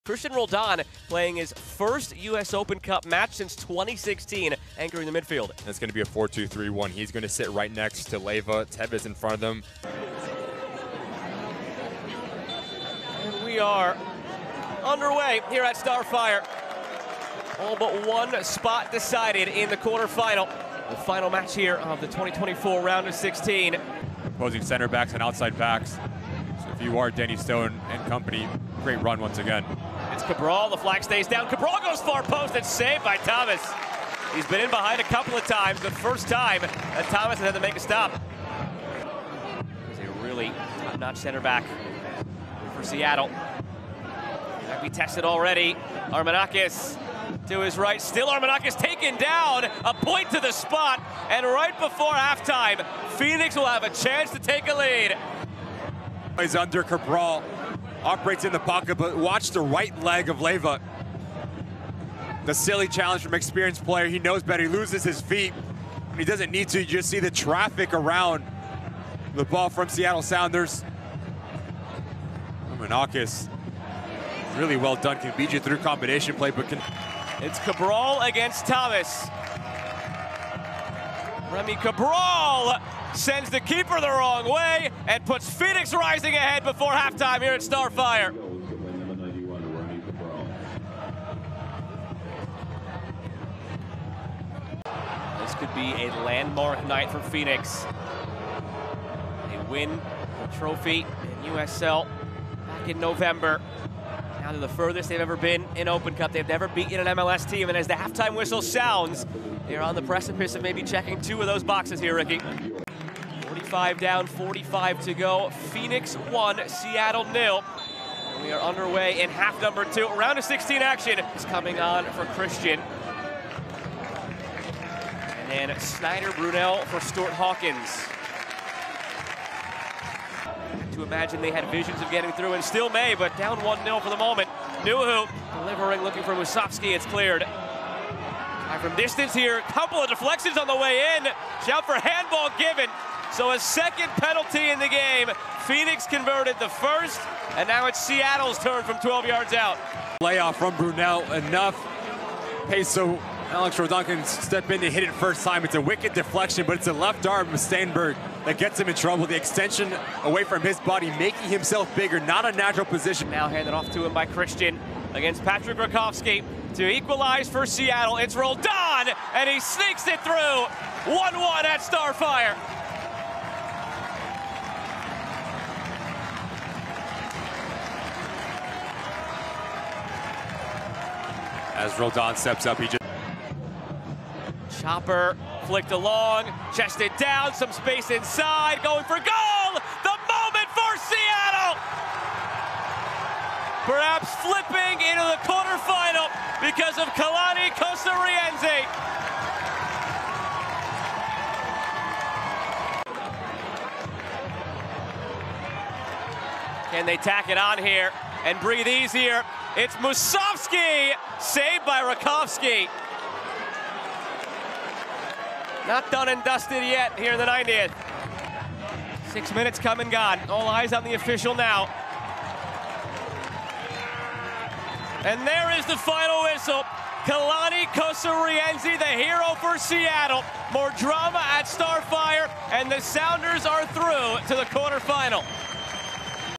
HIGHLIGHTS: Seattle Sounders FC vs. Phoenix Rising | May 22, 2024
The Seattle Sounders defeated Phoenix Rising FC of the USL Championship 2-1 on Wednesday night at Starfire Sports Complex in Tukwila, Wash., in the Round of 16 of the Lamar Hunt U.S. Open Cup.